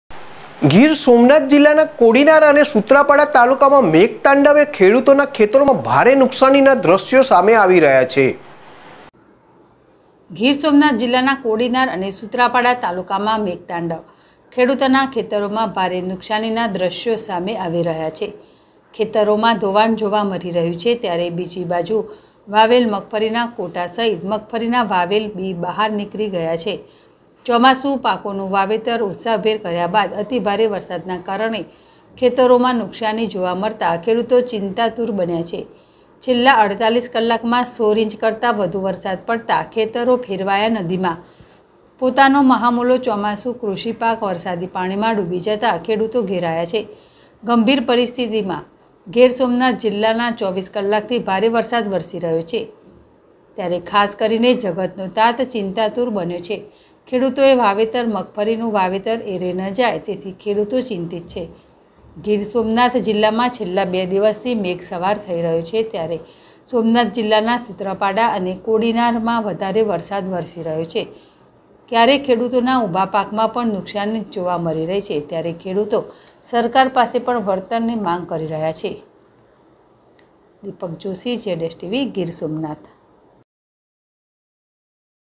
સોમનાથ જિલ્લાના કોડીનાર અને સુત્રાપાડા તાલુકામાં મેઘ તાંડવે વેર્યો ક્યારે ખેડૂતો ના ખેતરો
સોમનાથ જિલ્લાના કોડીનાર અને સુત્રાપાડા તાલુકામાં મેઘ તાંડવે વેર્યો ક્યારે ખેડૂતો ના ખેતરો માં ભારે નુકસાની ના દ્રશ્ય સામે આવી રહ્યા છે